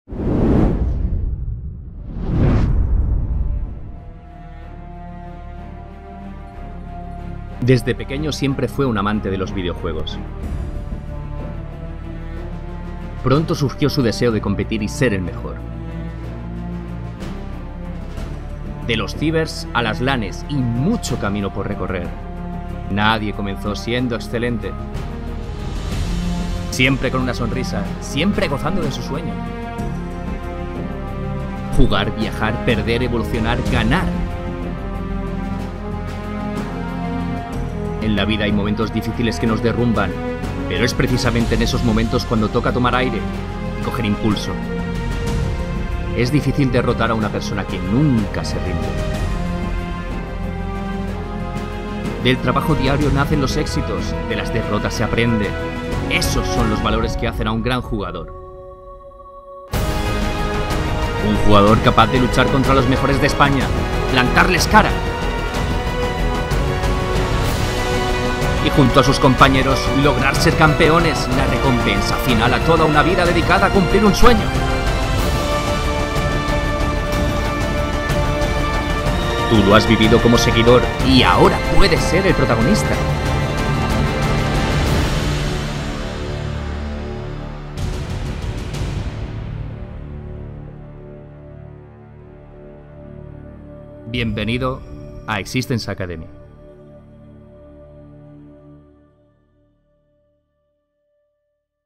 Presentamos la x6tence Academy
Publicado el 22 feb. 2017
👽 BIENVENIDOS AL CANAL DE YOUTUBE DE X6TENCE👽

Para entrar a la academia se organizará un torneo basado en 4 clasificatorios, uno por semana, algunos entre semana y otros en fin de semana para adaptarse a los horarios lo máximo posible.

De cada clasificatorio saldrán 2 equipos, en total 8, para disputar un playoff final.

El club dispondrá de ojeadores para poder fichar a jugadores sueltos que disputen los clasificatorios. No solo ganando el torneo tenéis opciones de entrar a la academia, esforzaros para jugar lo mejor posible!

La normativa se anunciara en la pagina de una vez se hayan abierto las inscripción.

La fecha donde se abren las inscripciones se anunciara en las RR.SS del club y de TGX.